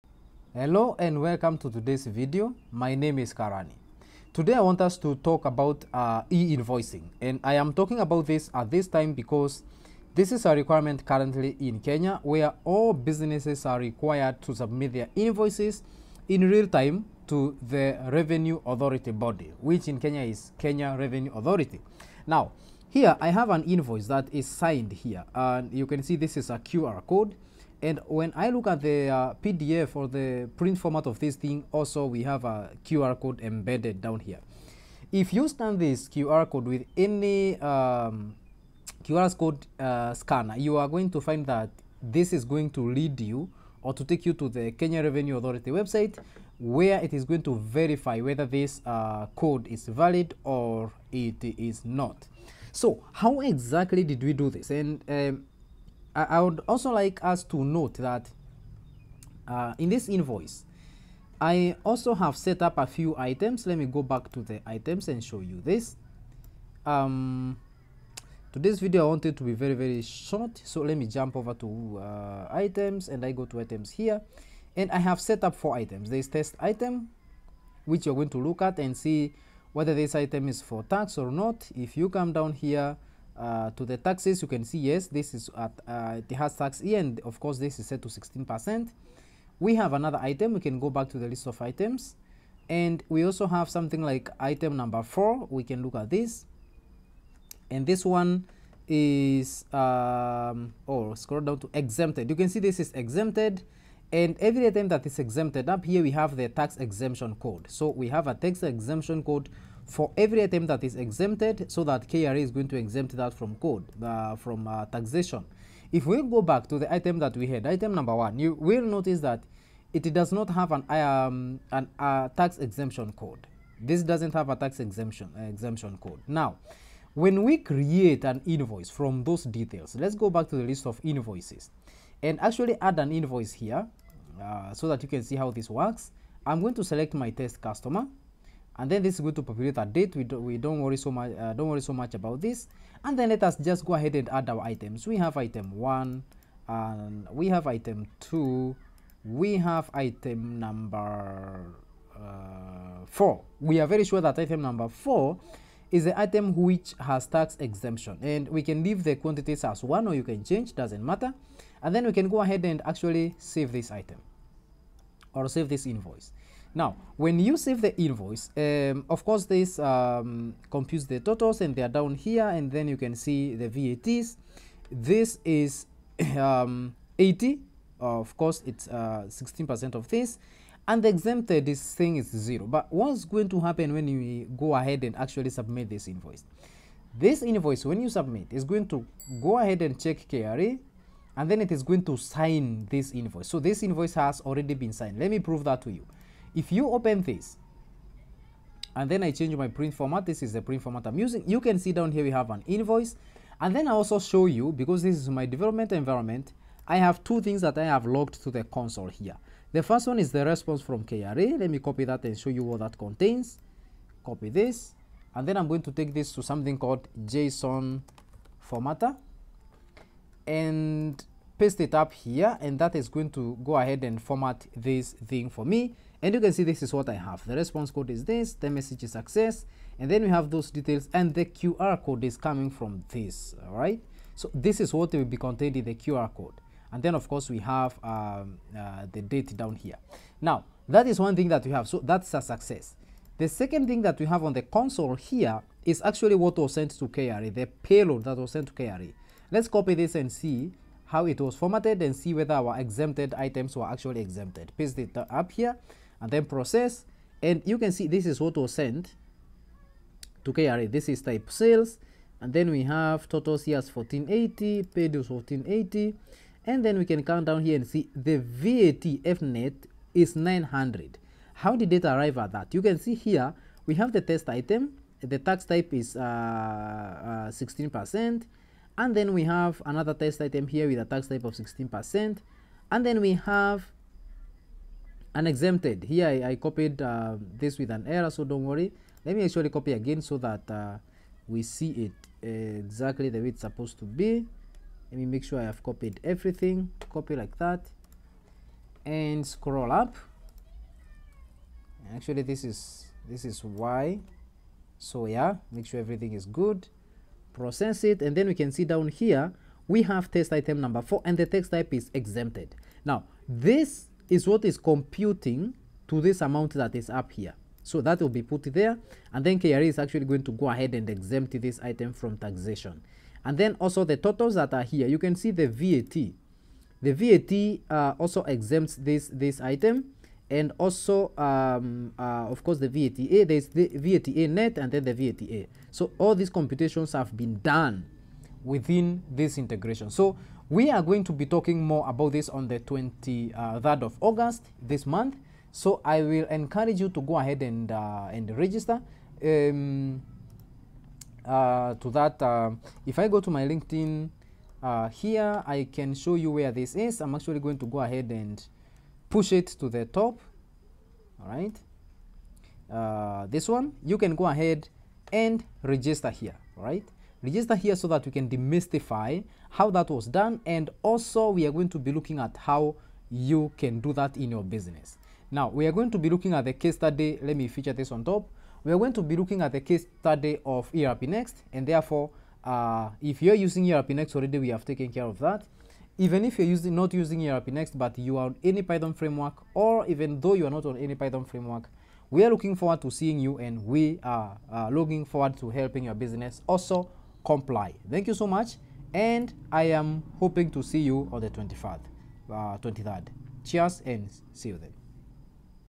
E-Invoicing in ERPNext and the Frappe Framework
einvoicing is new in kenya, and kra requires businesses to comply by September 30th. At upeosoft, we are helping businesses with integration and the automated signing of documents with the gadgets kra has approved. Join us on a virtual call on August 23rd as we demystify the requirements.

    • Username: upeosoft
    • Password: Upeosoft